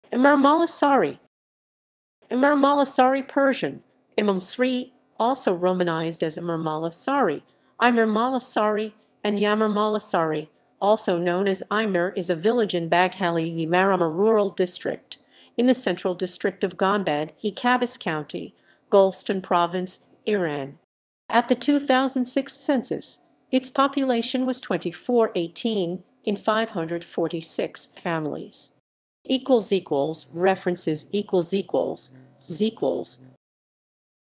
Imer Molla Sari
Imer Molla Sari (Persian: ايمرملاساري‎, also Romanized as Īmer Mollā Sārī, Eymar Mollā Sārī, and Yamar Mollā Sārī; also known as Eymar) is a village in Bagheli-ye Marama Rural District, in the Central District of Gonbad-e Qabus County, Golestan Province, Iran. At the 2006 census, its population was 2,418, in 546 families.

== References ==